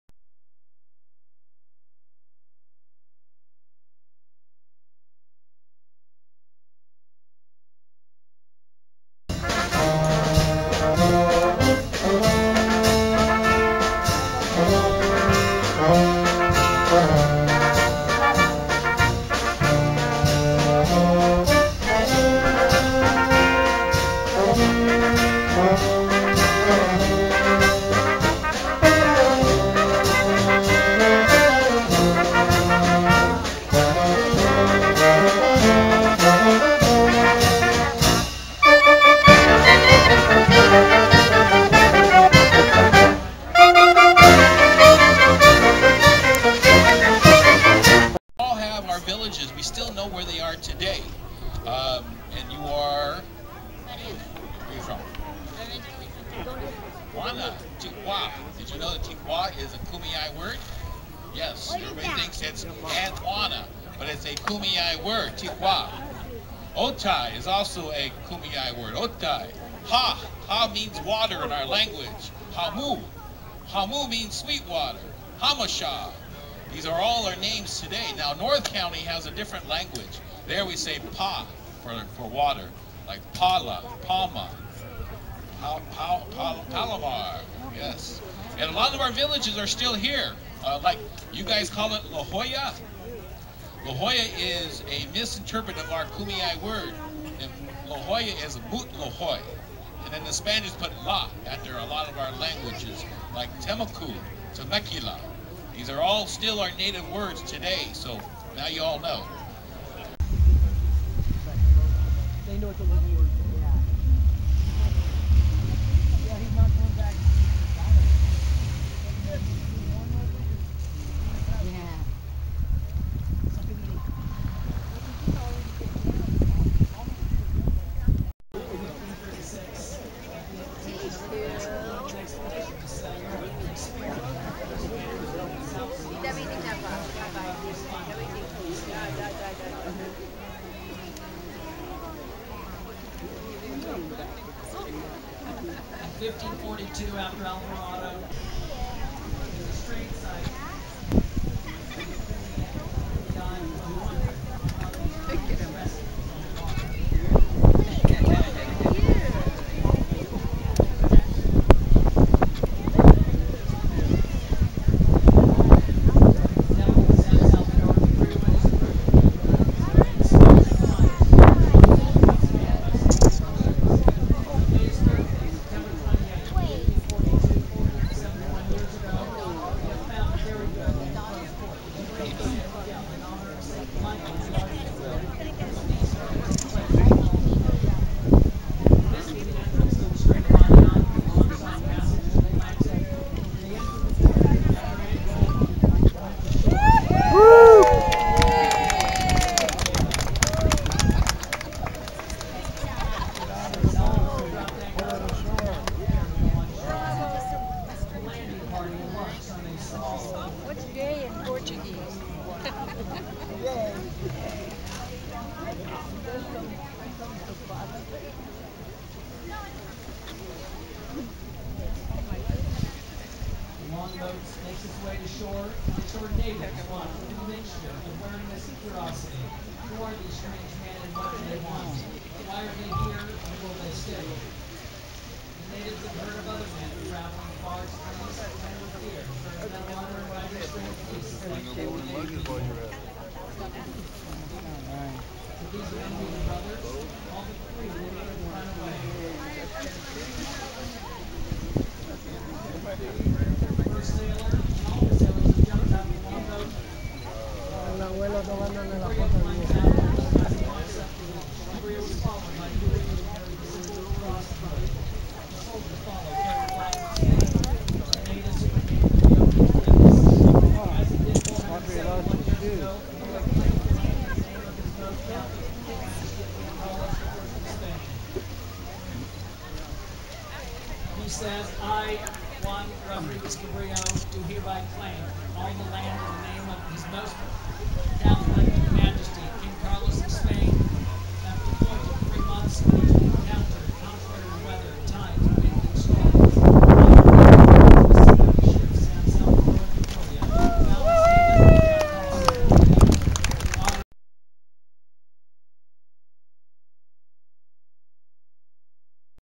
Cabrillo Festival 2013
9/29/13: Sights and sounds of the fiftieth annual San Diego Cabrillo Festival, including a re-enactment of Juan Rodriguez Cabrillo's 1542 landing at Ballast Point, the first visit by a European to California. Also in the video are the Portuguese Philharmonic Band of San Diego, and Native American storyteller Running Grunion.